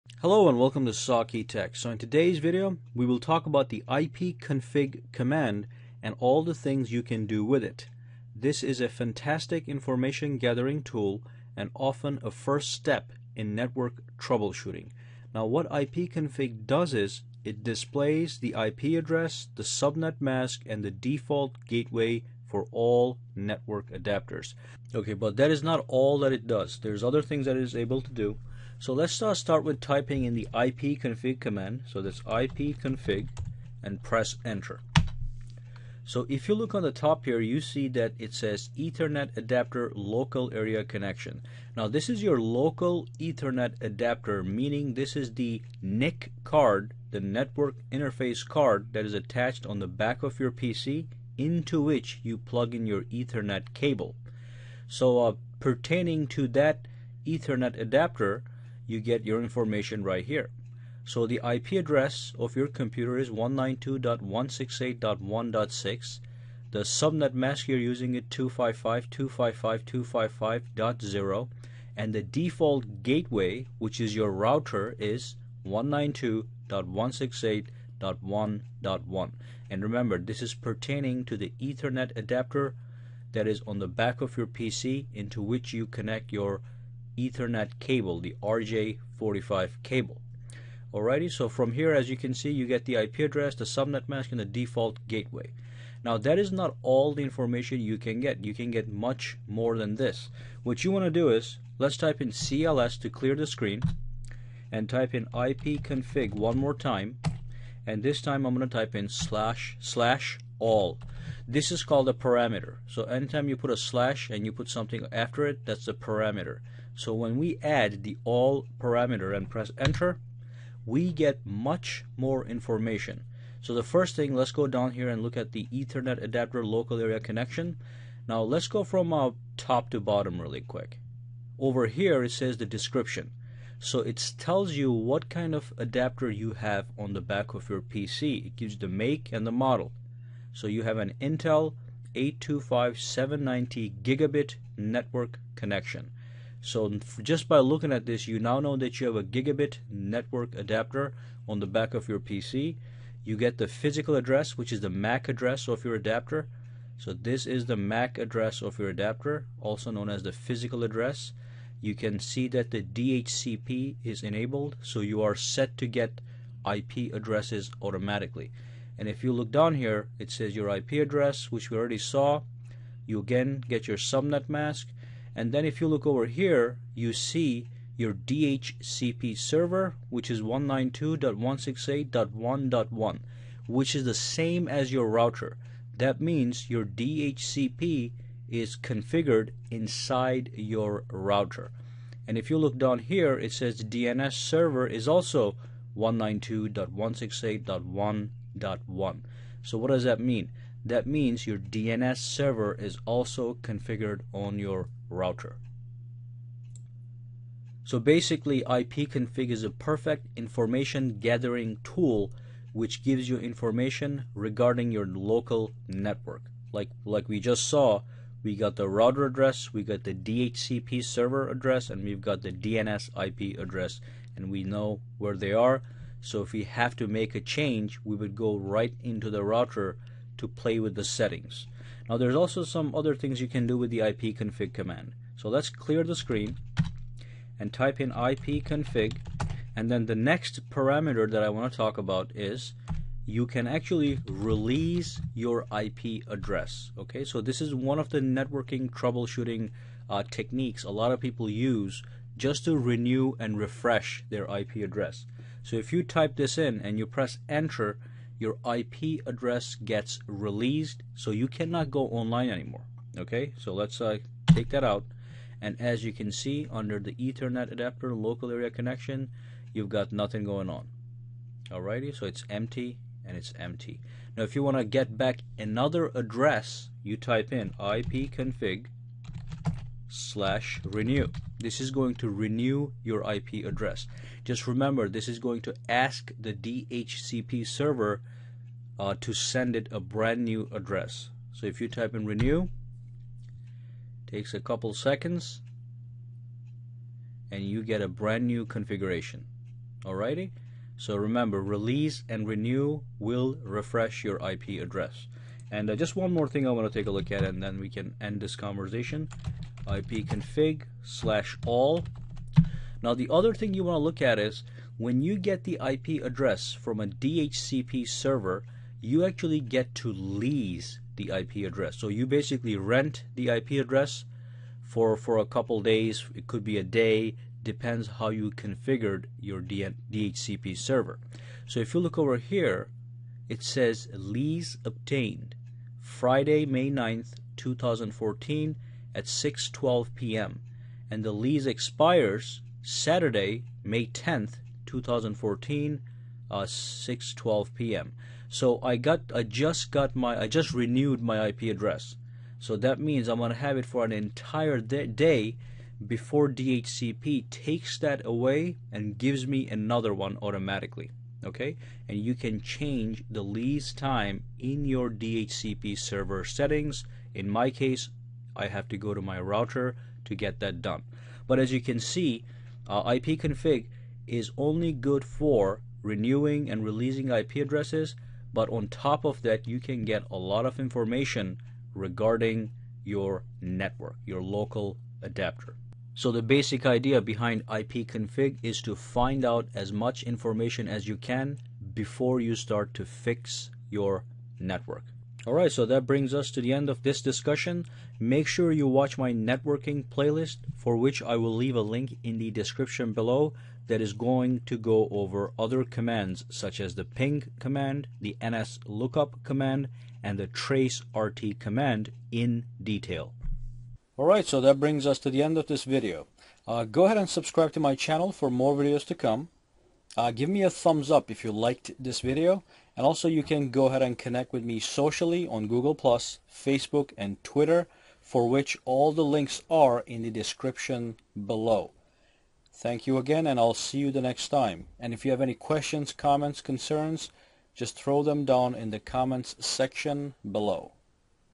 Network Troubleshooting using the IPCONFIG Command
In this tutorial, I will do an in-depth look at the ipconfig command for the purposes of network troubleshooting. ipconfig command is a fantastic utility to find out as much information as possible about your local area network before you start to troubleshoot your network.

We will go over:

ipconfig
ipconfig _all
ipconfig _release
ipconfog _renew

FOLLOW ME